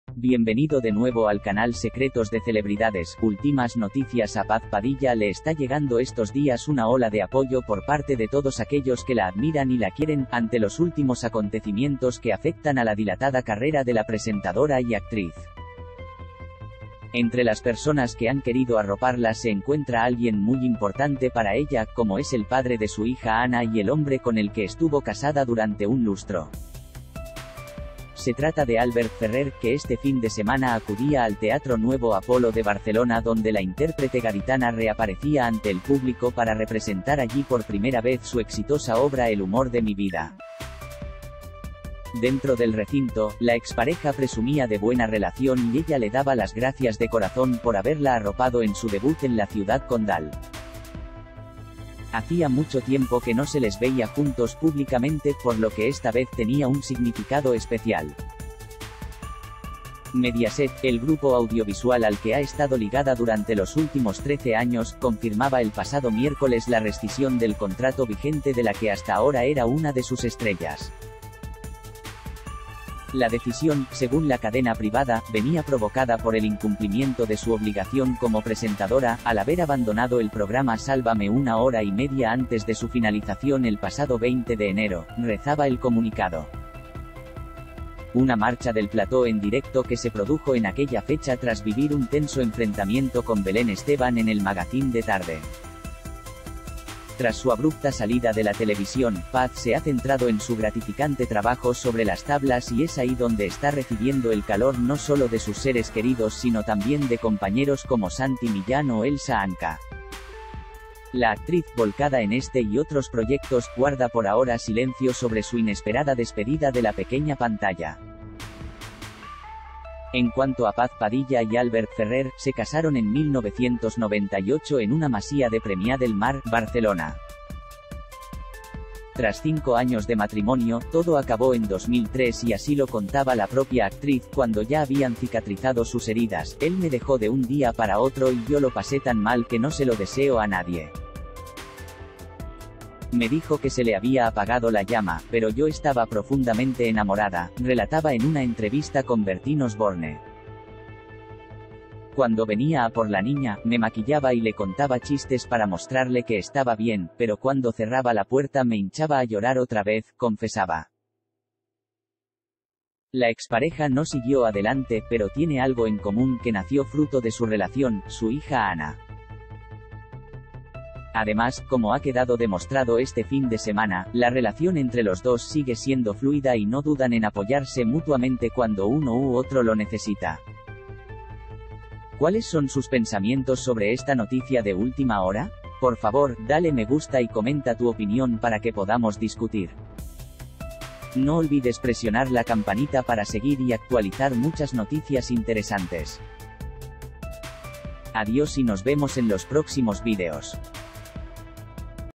Paz Padilla recibe el cariño de su ex, padre de su hija Anna Ferrer, en su reaparición ante el públi
Paz Padilla recibe el cariño de su ex, padre de su hija Anna Ferrer, en su reaparición ante el públi. APaz Padilla le está llegando estos días una ola de apoyo por parte de todos aquellos que la admiran y la quieren, ante los últimos acontecimientos que afectan a la dilatada carrera de la presentadora y actriz. Entre las personas que han querido arroparla se encuentra alguien muy importante para ella, como es el padre de su hija Anna y el hombre con el que estuvo casada durante un lustro.